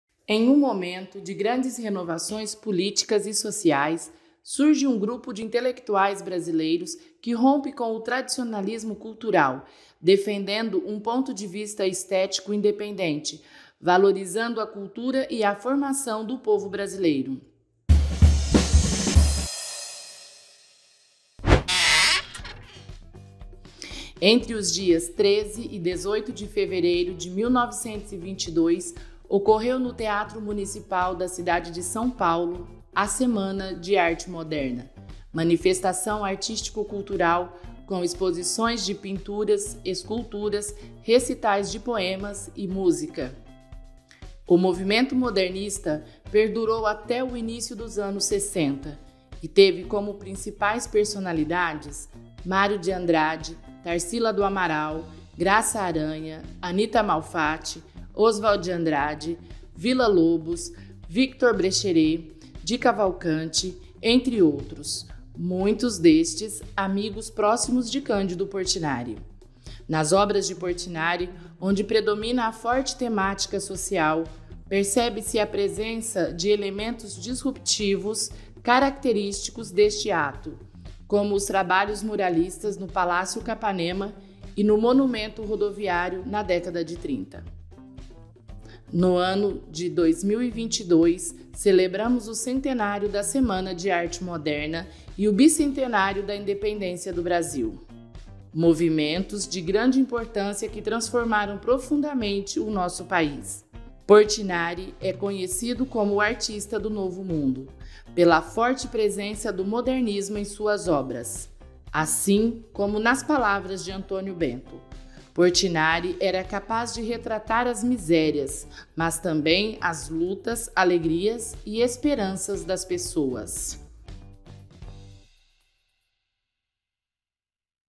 Pela Janela - Especial Centenário da Semana de Arte Moderna de 1922
Quer conhecer um pouquinho sobre a Semana de Arte Moderna de 22? A Equipe Educativa do Museu Casa de Portinari conta aqui no vídeo. Play pra aprender!

É muito importante sabermos sua opinião!
Responda a pesquisa acessando o link, contribuindo assim para a constante evolução dos conteúdos do Museu Casa de Portinari.